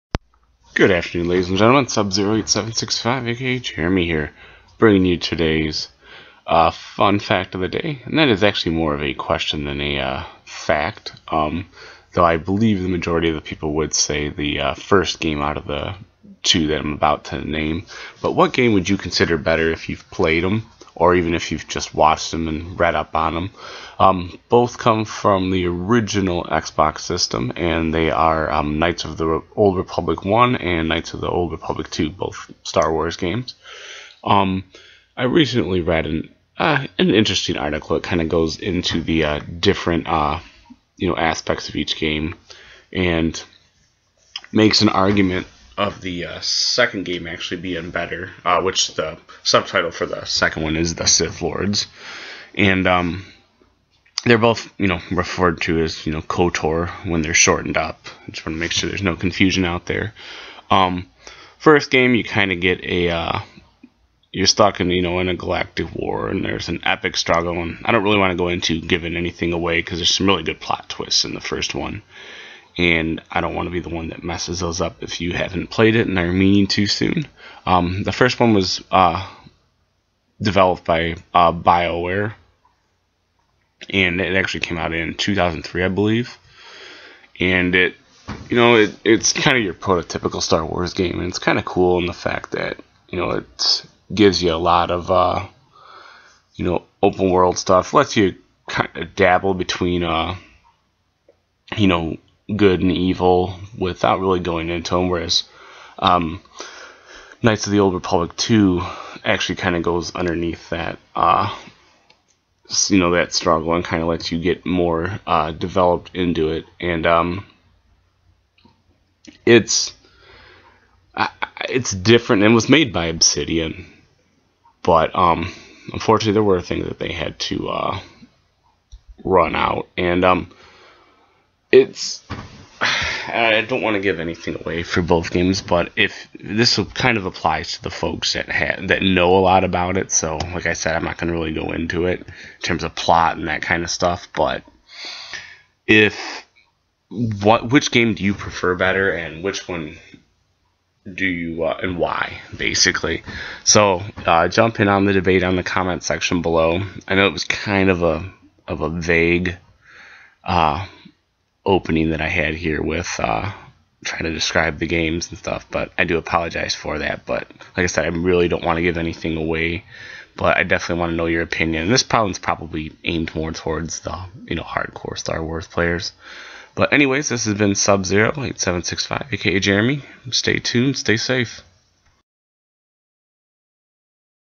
Fun Fact of the Day Which Knights of the Old Republic Game is Better
Here is a question for you folks that have played Knights of the Old Republic 1 or Knights of the Old Republic 2 Sith Lords for the Xbox?